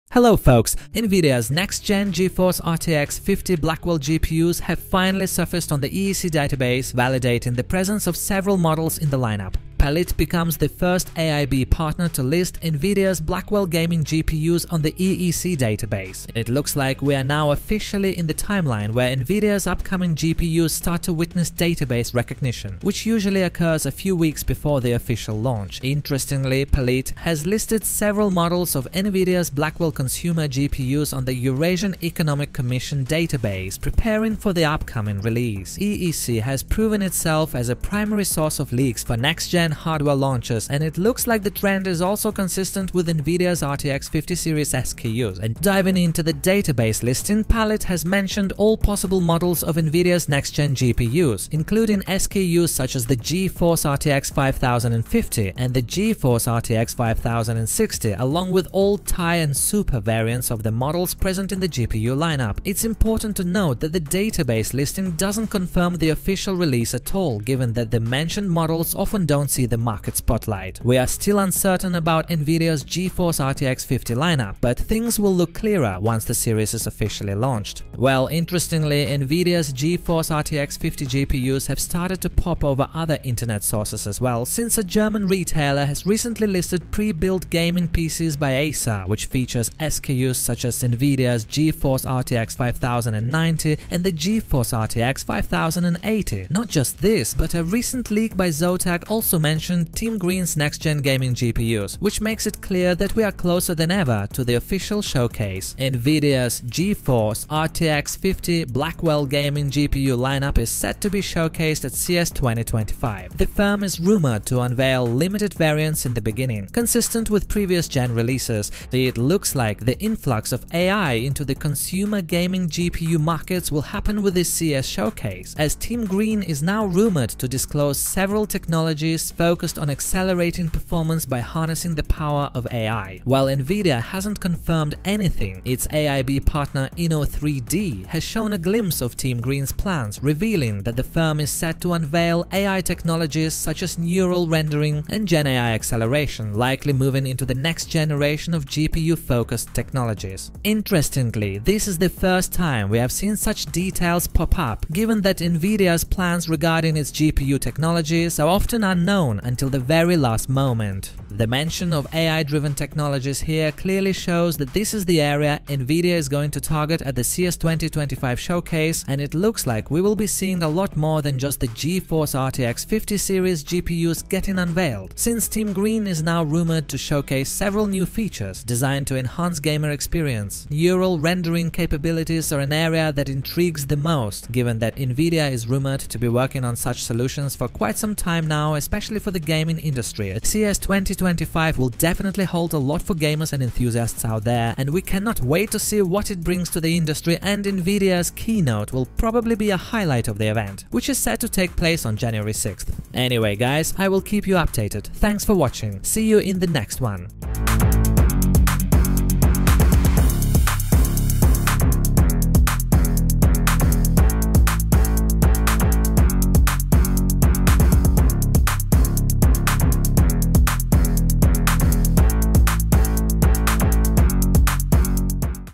NVIDIA GeForce RTX 50 Series - ALL MODELS OFFICIAL!!!
NVIDIA's next-gen GeForce RTX 50 "Blackwell" GPUs have finally surfaced on the EEC database, validating the presence of several SKUs in the lineup.